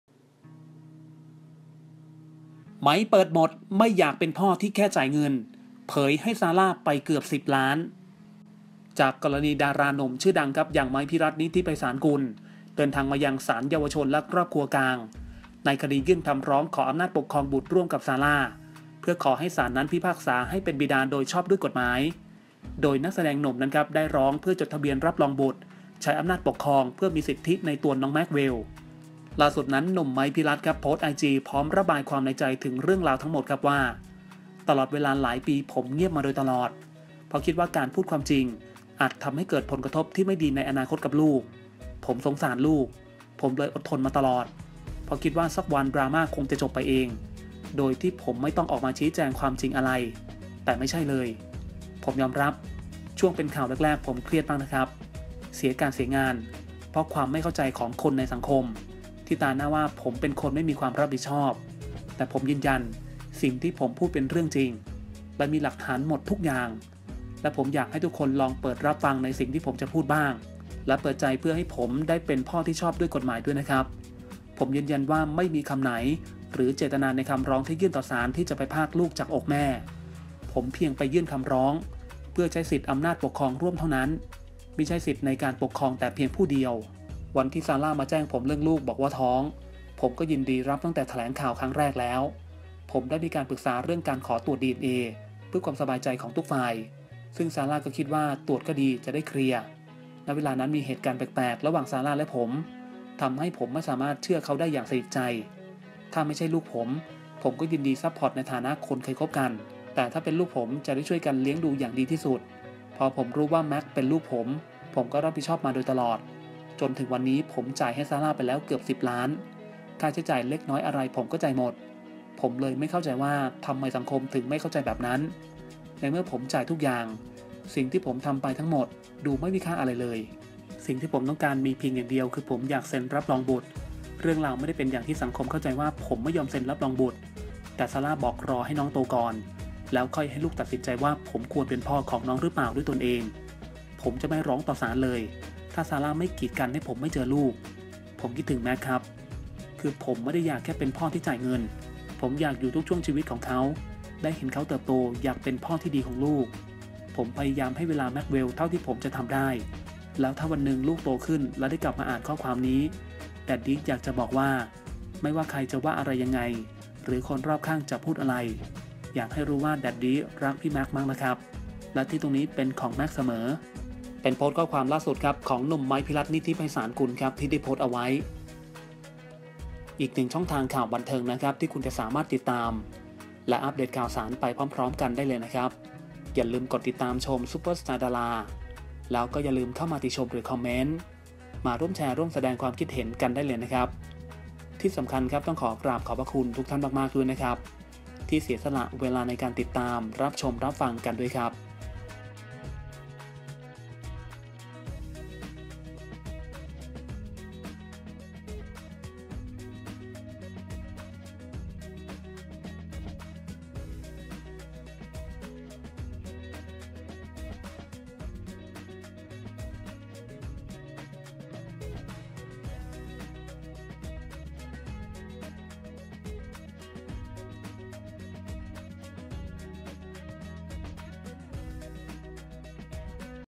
ไมค์ เปิดหมด! ไม่อยากเป็นพ่อที่แค่จ่ายเงิน เผยให้‘ซาร่า’ไปเกือบสิบล้าน#super star dara
เครดิต:: m1keangelo , sarahcasinghini 🙏

ไมค์ เปิดหมด! ไม่อยากเป็นพ่อที่แค่จ่ายเงิน เผยให้‘ซาร่า’ไปเกือบสิบล้าน
จากกรณี ดาราหนุ่มชื่อดัง ไมค์ พิรัชต์ นิธิไพศาลกุล เดินทางมายังศาลเยาวชนและครอบครัวกลาง ในคดียื่นคำร้องขออำนาจปกครองบุตรร่วมกับซาร่า เพื่อขอให้ศาลพิพากษาให้เป็นบิดาโดยชอบด้วยกฎหมาย